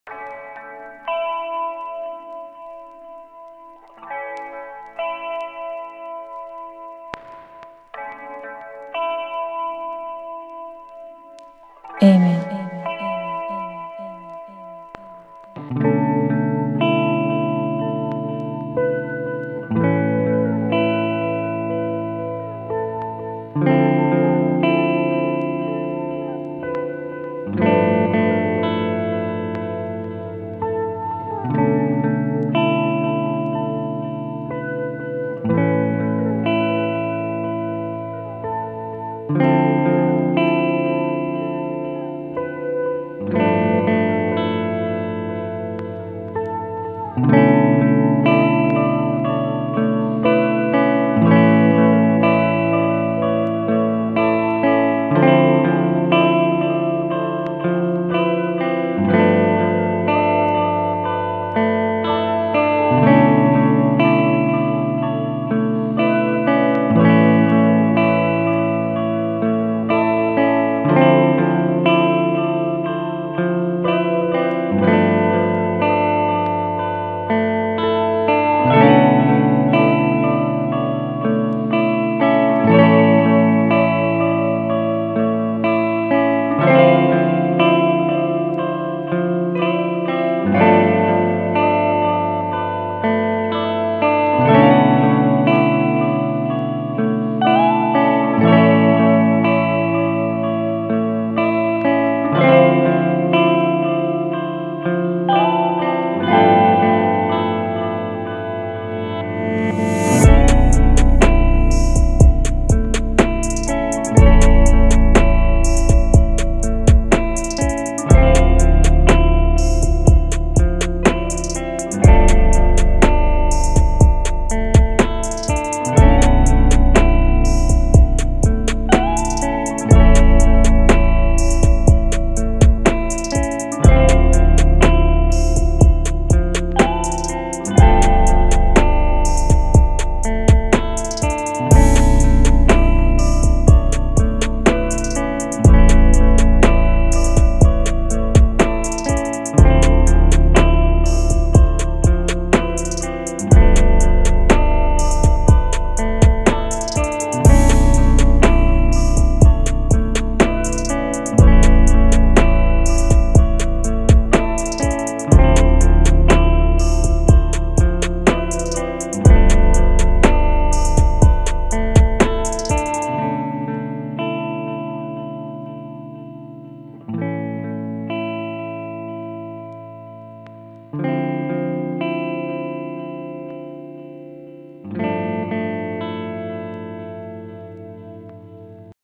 [Free beat 무료비트] 감성적인 / guitar pink sweats type beat hiphop R&B instrument / For - Hoodbeats / 2019
For - Hoodbeats
Prod. nemA
Chill mood hiphop Beat

비상업적 무료사용 가능하며
이용시에는 아래 메일로 반드시 먼저 연락주시고  Prod. nemA를 붙여주세요
댓글 달아주시면 비트 보내드립니다 !

You can be used free of charge for non-commercial purposes.
Please attach 'Prod by nemA' when using it.